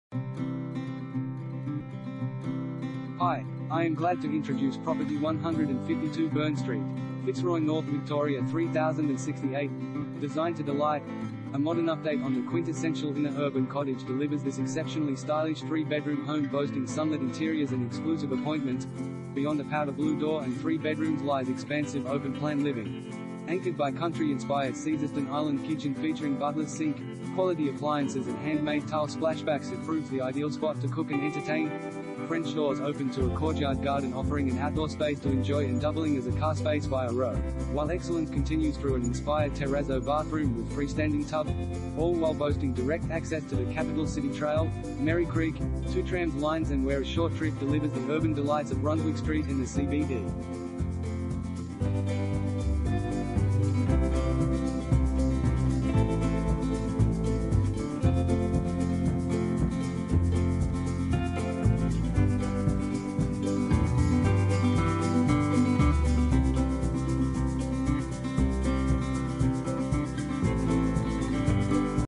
152 Byrne Street, Fitzroy North VIC 3068 | AuPropertyReport.Com
152 Byrne Street, Fitzroy North VIC 3068
Property Type: house
Bedrooms: 3
Bathrooms: 3
Car Space: 1
Designed to delight